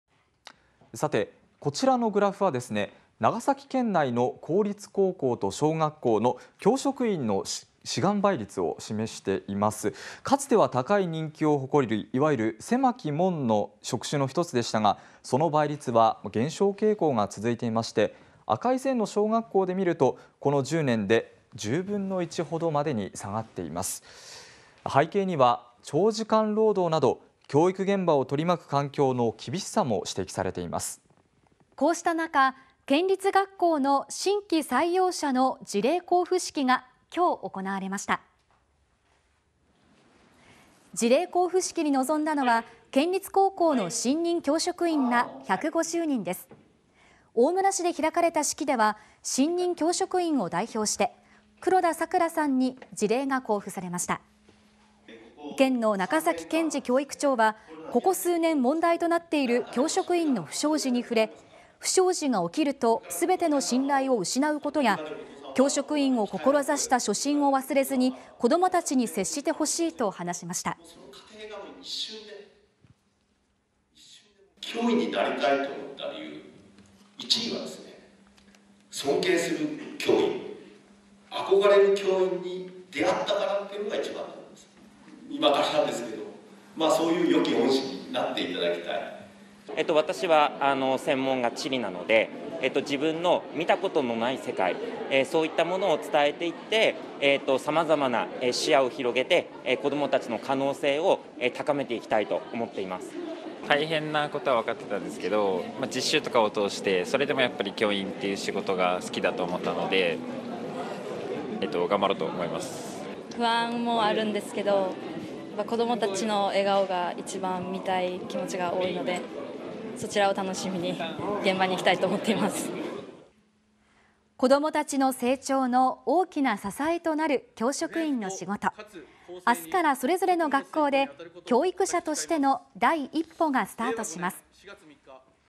「不祥事で信頼を失う 教職員を志した初心を忘れずに」公立学校 教職員の新規採用者 辞令交付
公立高校と小学校の “教職員” は、かつては高い人気を誇り、いわゆる狭き門の職種の一つでしたが、長崎県内では採用試験の倍率の減少傾向が続いていて、小学校ではこの10年で10分の1ほどにまで下がっています。
この背景には『長時間労働』など教育現場を取り巻く環境の厳しさも指摘されています。こうした中、長崎県立学校の新規採用者の辞令交付式が3日、行われました。辞令交付式に臨んだのは、長崎県立高校の新任教職員ら150人です。大村市で開かれた式では、新任教職員を代表して 黒田さくらさんに辞令が交付されました。中崎 謙司 県教育長は、ここ数年 問題となっている “教職員の不祥事”に触れ、不祥事が起きると すべての信頼を失うことや「教職員を志した初心を忘れずに子ども達に接して欲しい」と話しました。
 長崎西高に赴任する新任教職員：
「私は専門が地理なので、生徒が見たことのない世界を伝えて視野を広げ子供たちの可能性を高めていきたい」猶興館高校に赴任新任教職員：
「大変なことはわかっていたんですけど、実習とかを通してそれでも教員という仕事が好きだと思ったので頑張ろうと思います」島原高校に赴任する新任教職員：
「不安もあるが子どもたちの笑顔を見たい気持ちが大きい。楽しみにして現場に行きたい」子ども達の成長の大きな支えとなる教職員の仕事。
4日からそれぞれの学校で、教育者としての第一歩がスタートします。